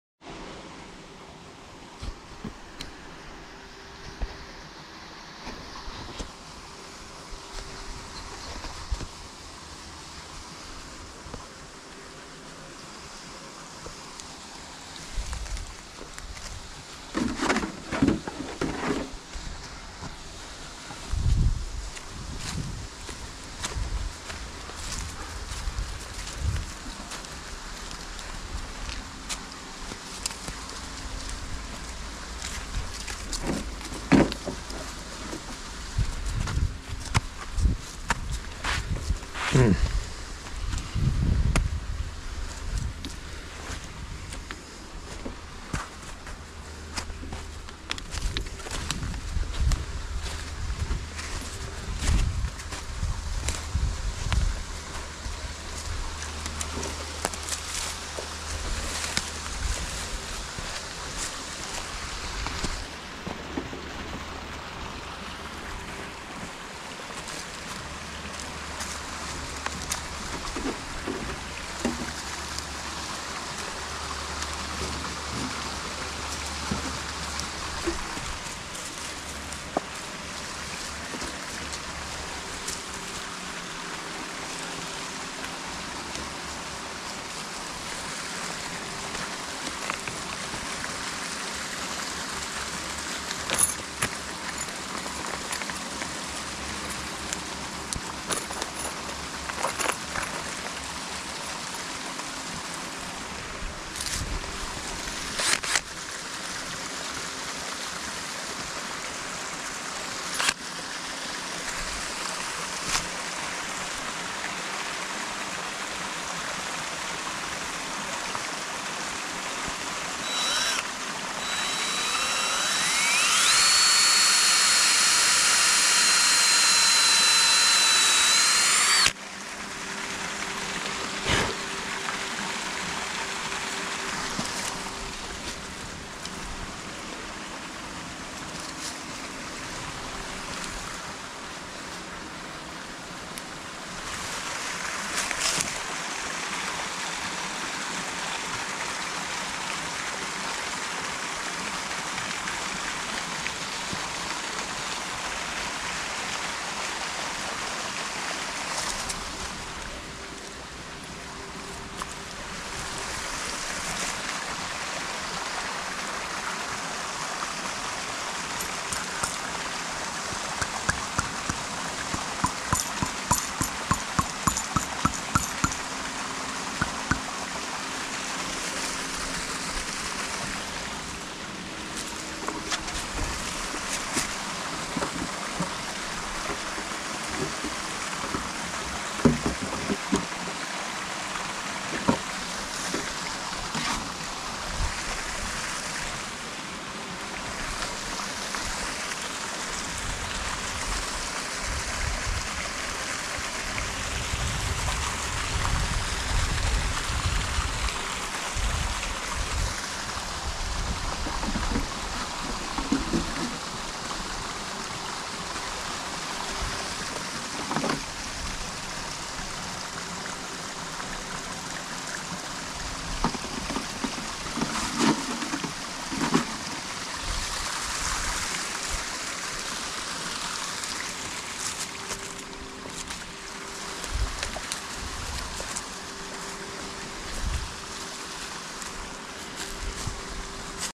First time tapping Black Maples; Part 1
First time ever trying to tap a tree; these are black maples in Pittsburgh, PA

Shot on a GoPro Hero 9 Black